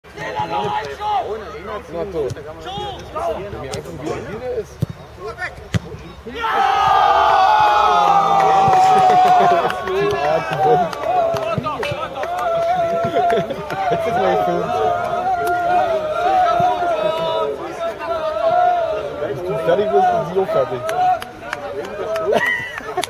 2009 08 06 Training beim FC Energie Cottbus mit Pele Wollitz
Der neue Geist beim Training des FC Energie Cottbus unter Pele Wollitz ...